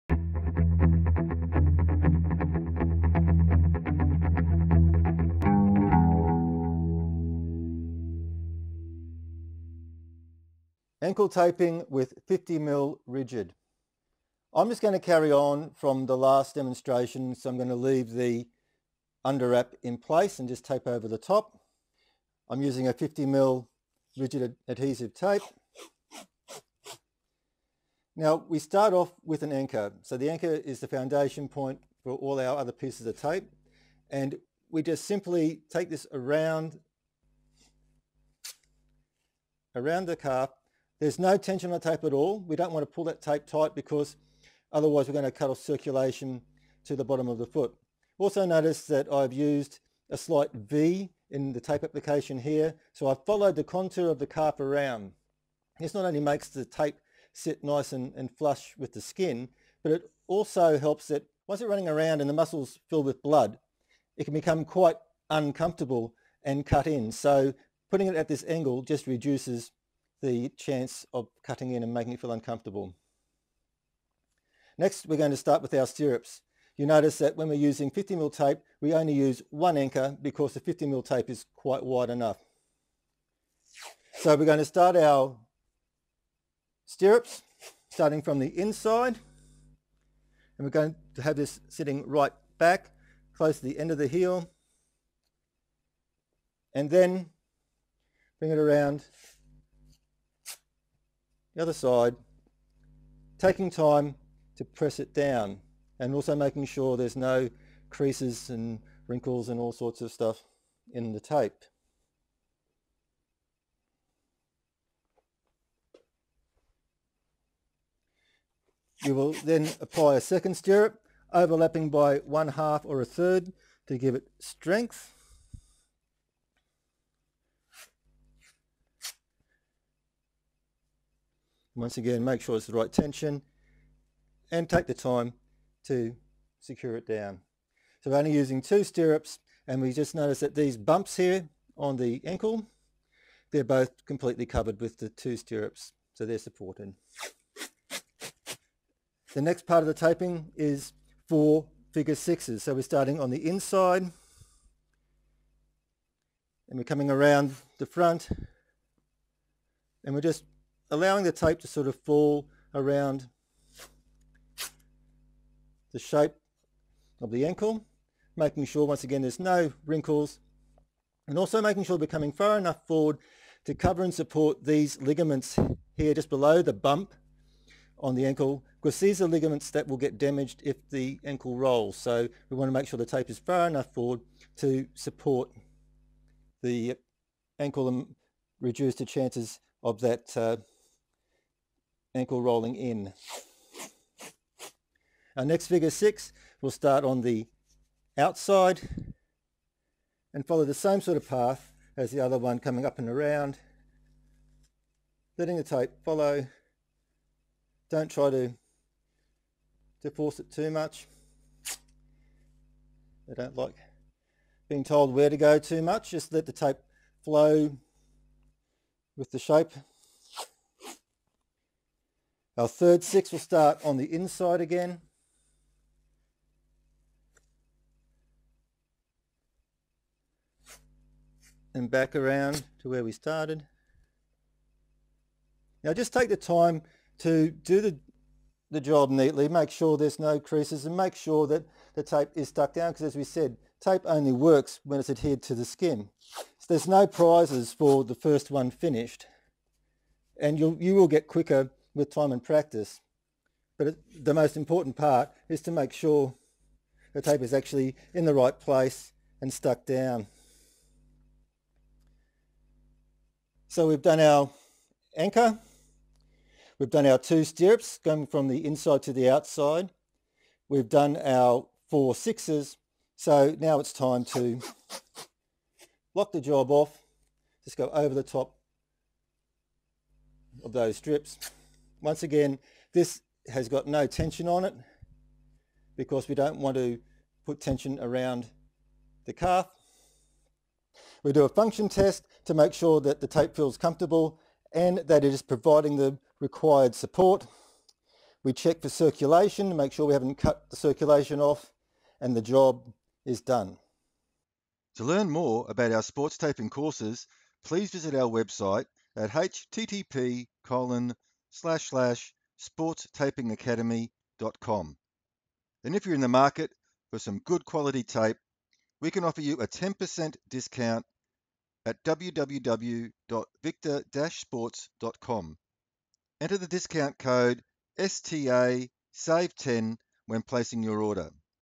How To Strap An Ankle Using 50mm Rigid Sports Tape
Are you a coach, team manager, club administrator or simply a mum or dad with a child in sport? Are you interested in learning more about how to tape to reduce the chances of injury?

Preventative ankle taping is the same regardless of the sport and is something anyone can master with training, time and practice.

Football, netball, basketball, hockey, the same taping principles apply.

today to find out more about of comprehensive, easy to follow, step by step, online sports taping courses.